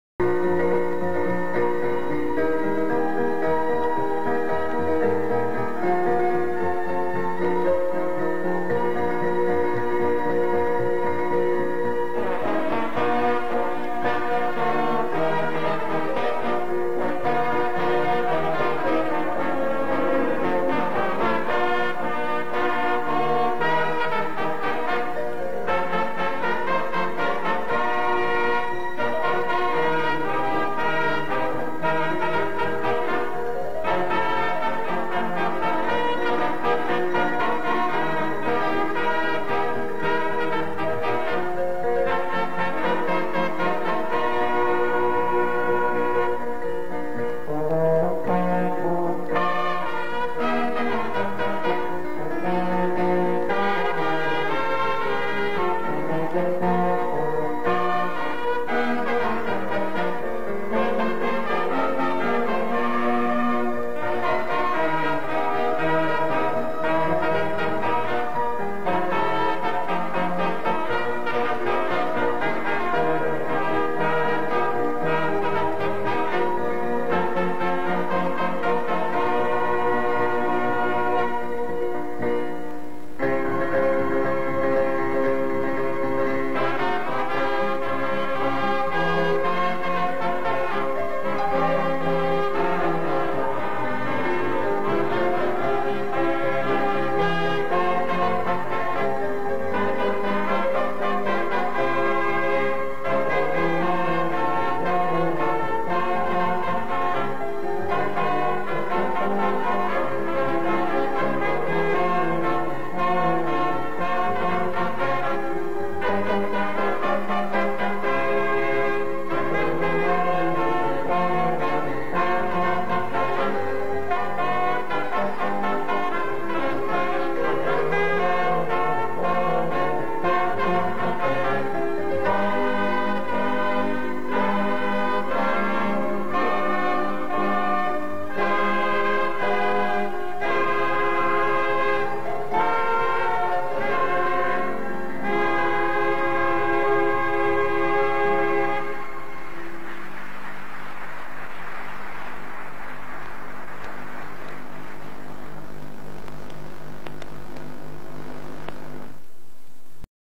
God's Choir - Brass ensemble Youth choir concert 2000
A musical concert performance of Deeper life youth choir in 2000